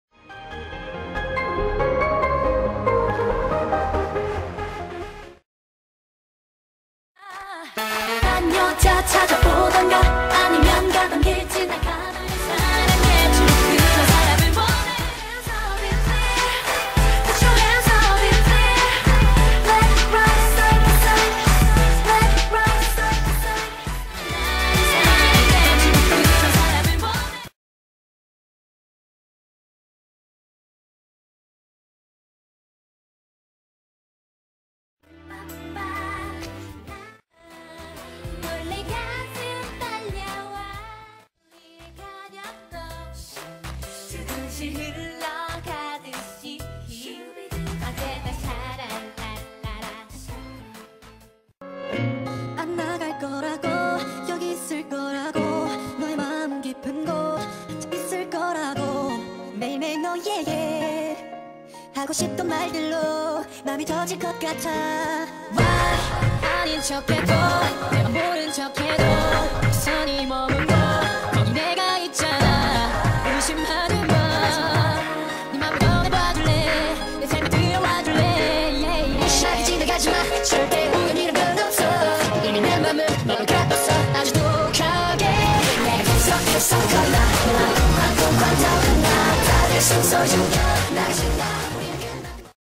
KNK Predebut Compilation
Crap I fell for KNK, help...Oh and also, I'll accept requests to scavenge for predebut stuff for any group

Here's a bunch of clips/photos of KNK during their Predebut days. If you haven't gotten into KNK yet, you should. Such total dorks. My biases are Inseong and Heejun.